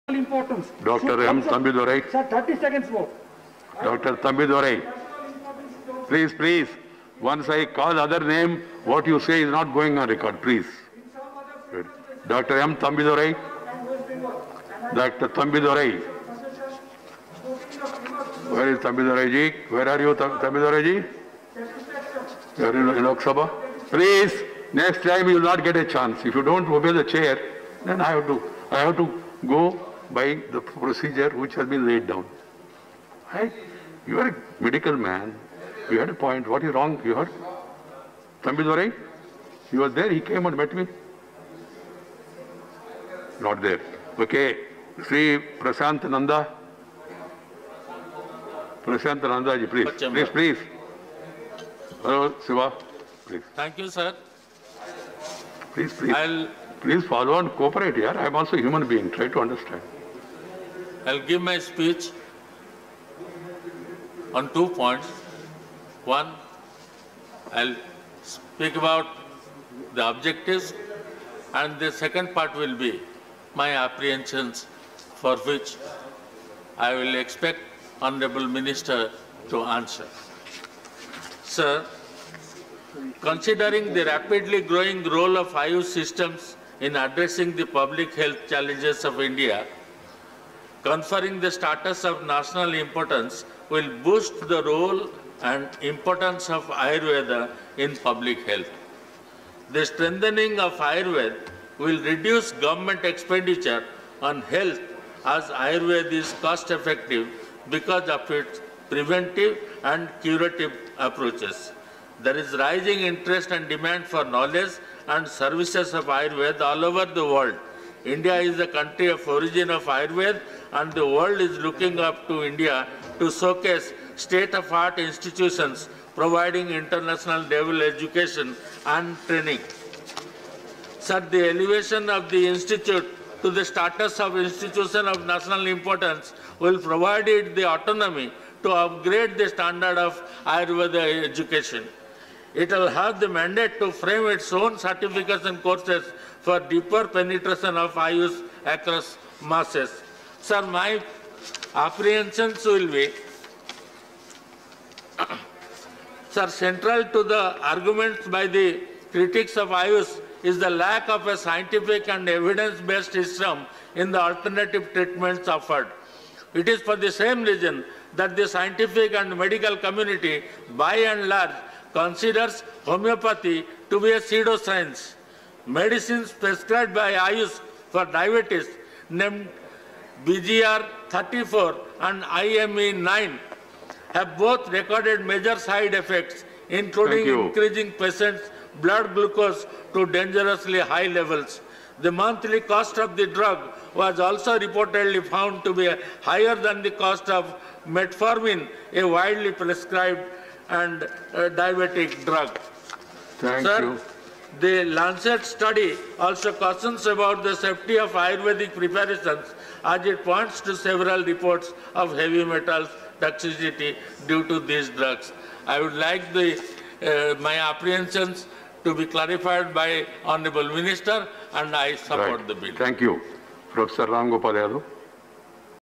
Prasantha Nanda's Remarks | Institute of Teaching & Research in Ayurveda Bill, 2020
Discussion on Institute of Teaching and Research in Ayurveda Bill, 2020

Bill seeks to provide for the establishment of an Institute of Teaching and Research in Ayurveda and to declare it as an Institution of national importance for the promotion of quality and excellence in education, research and training in Ayurveda and allied disciplines and for matters connected therewith or incidental thereto, as passed by Lok Sabha, be taken into consideration.